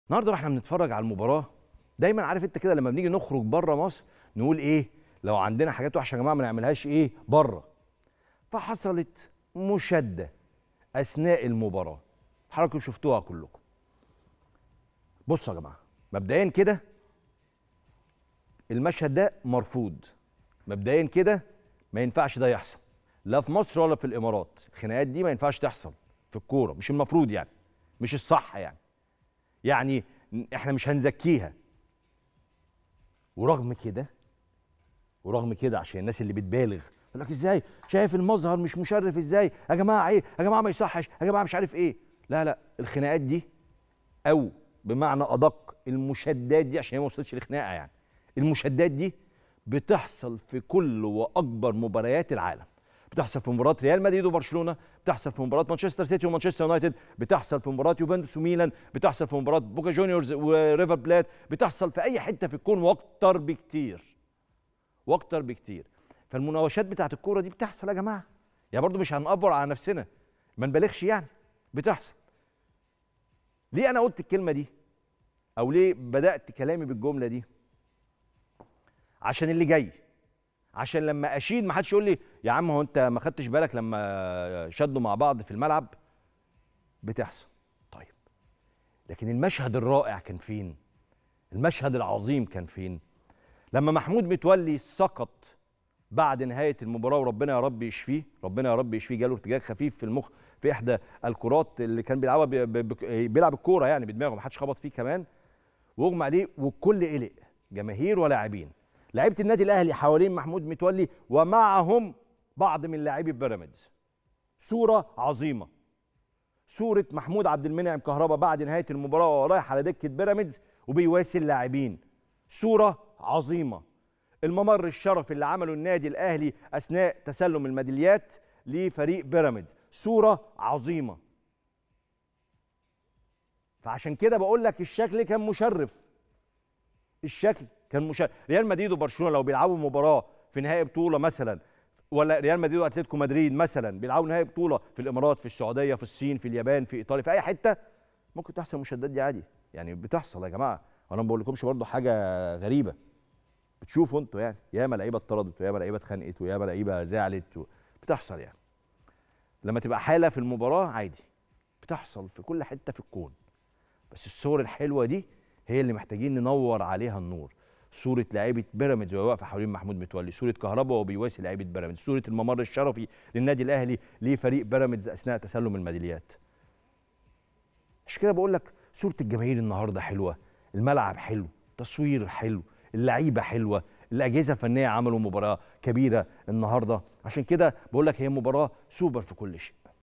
“ مشهد مرفوض ومشهد رائع 💥“ .. كريم رمزي يعلق على المشادة اللي حصلت في مباراة الأهلي وبيراميدز 🔥🔥
“ مشهد مرفوض ومشهد رائع 💥“ .. كريم رمزي يعلق على المشادة اللي حصلت في مباراة الأهلي وبيراميدز في نهائي السوبر 🔥🔥